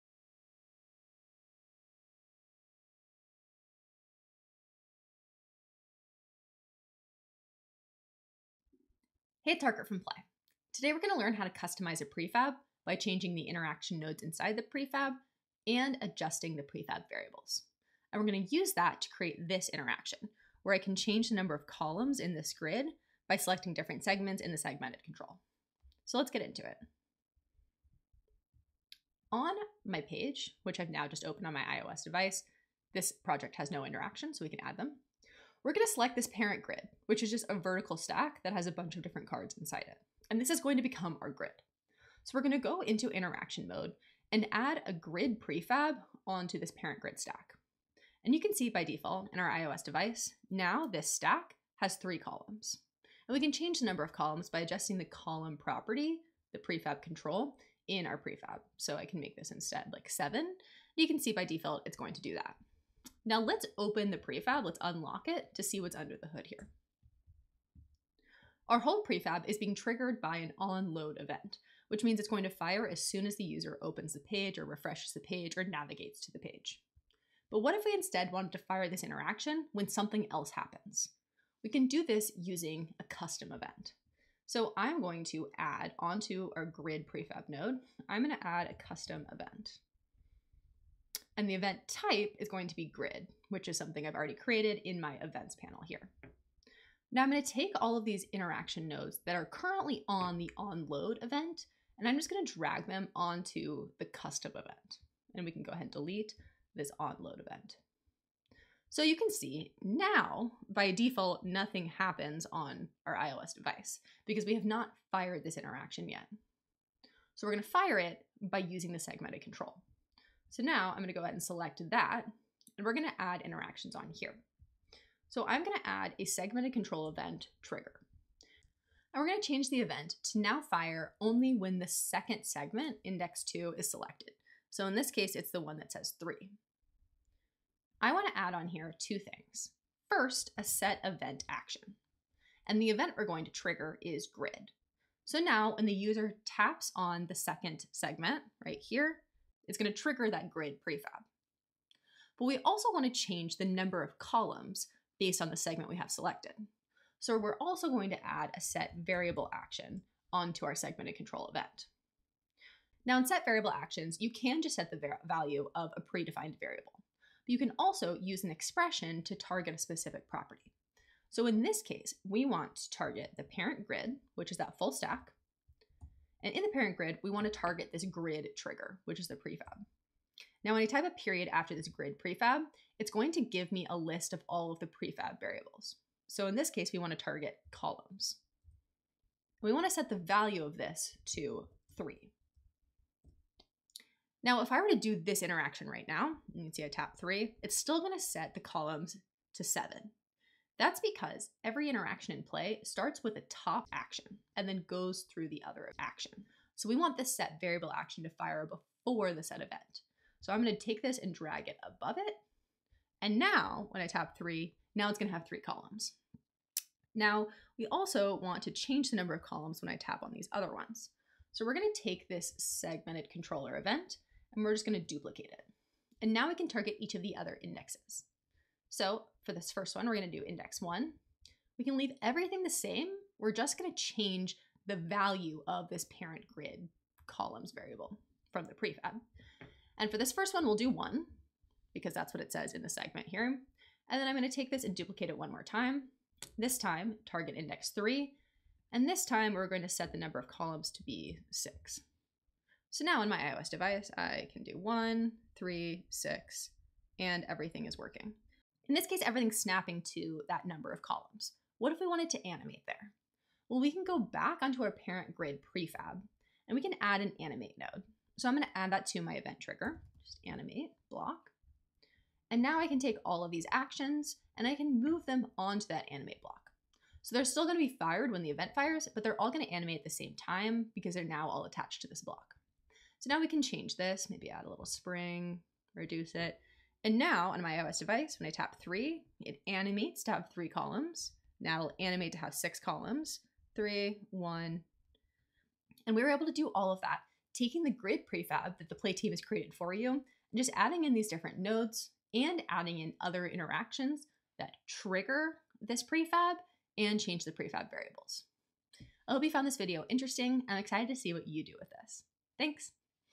Customizing a Prefab to Create a Dynamic Grid
In this video, you'll learn how to customize prefabs by changing the prefab's interaction nodes and adjusting the prefab variables through other interactions. We'll use a custom event, Segmented Controller Event triggers, and Set Event and Set Variable actions.